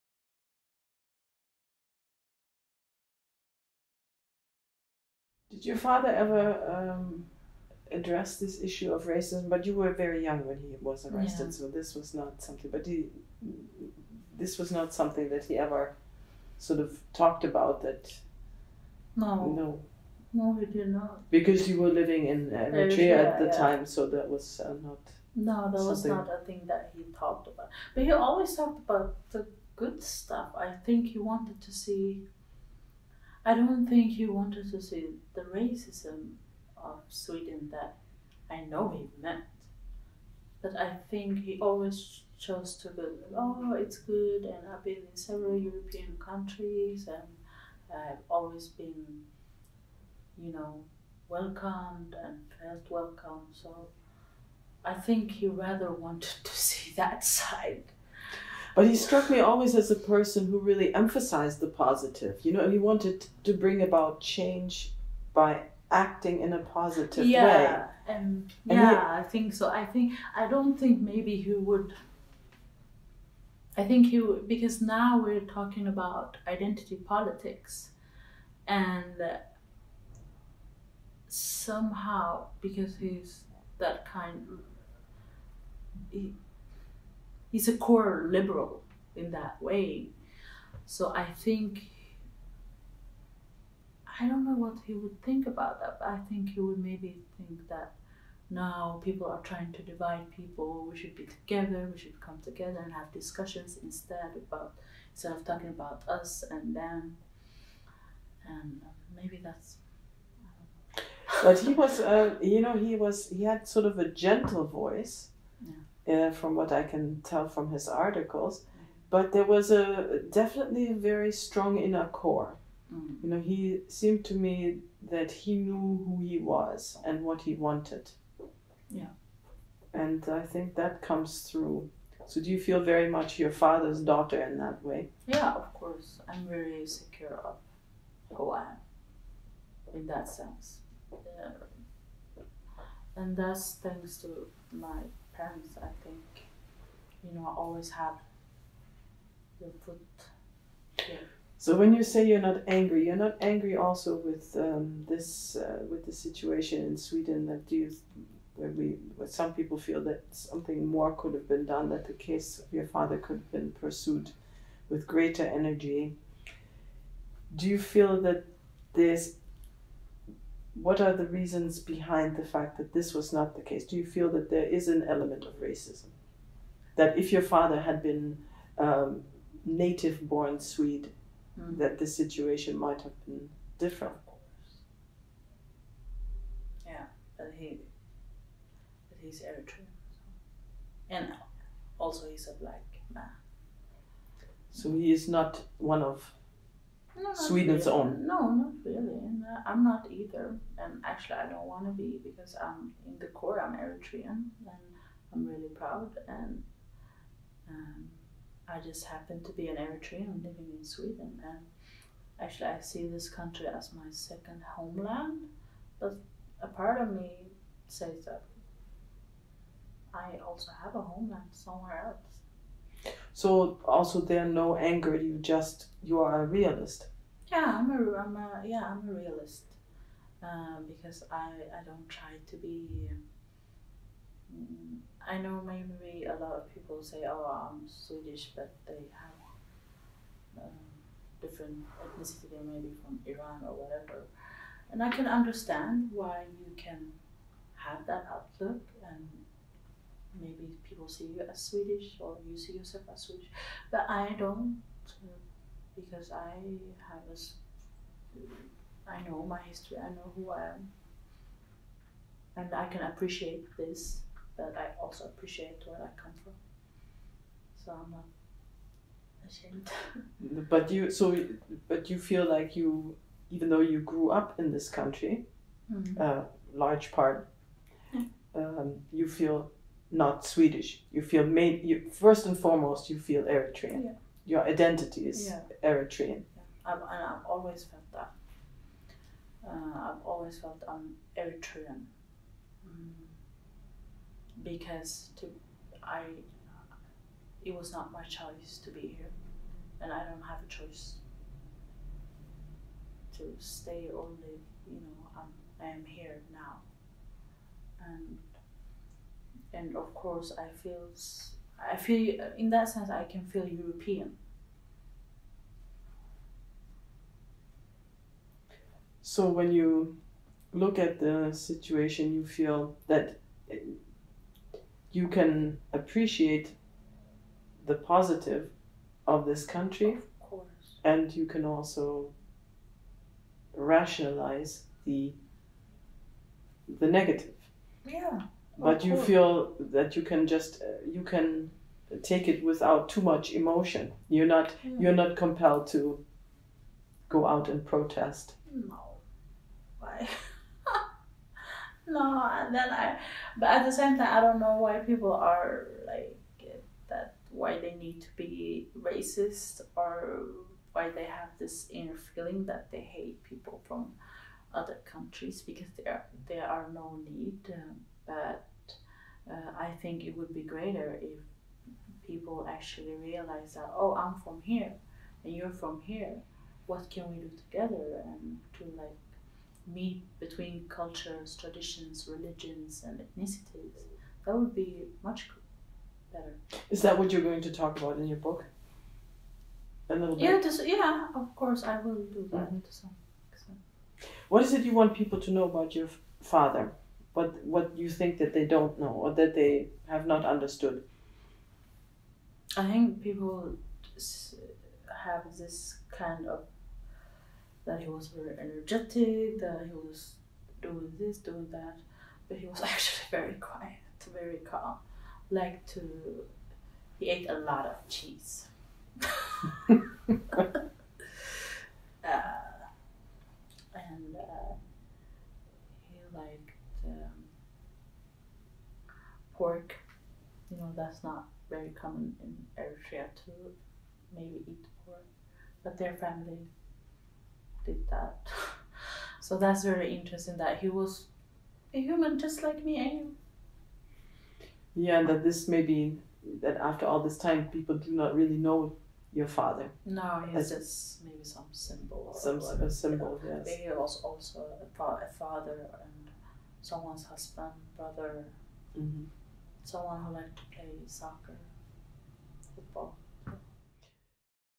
Interviewausschnitt: Betlehem Isaak - Betlehem about her father Dawit Isaak (Teil 3)
Bethehem Isaak, Aktivistin und Autorin, spricht über ihr Leben und die Arbeit ihres Vaters Dawit Isaak.